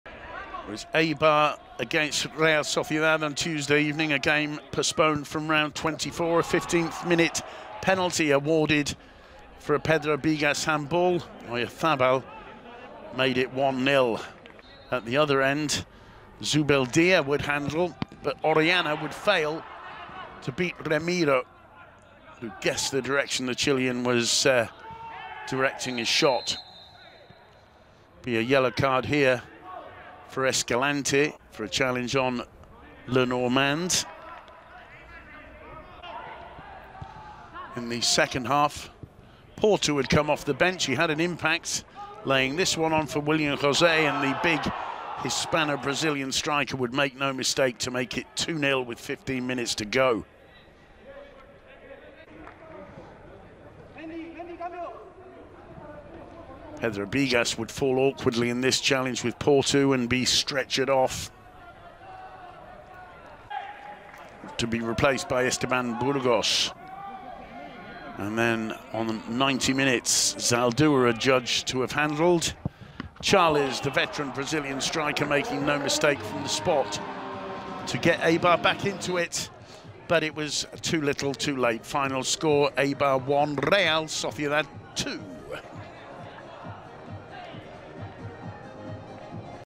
Highlights SD Eibar vs Real Sociedad (1-2)
Real Sociedad beat SD Eibar thanks to goals of Oyarzabal and William José. Charles, with a penalty goal, scored the goal of the local team. Matchday 24 LaLiga Santander 2019/2020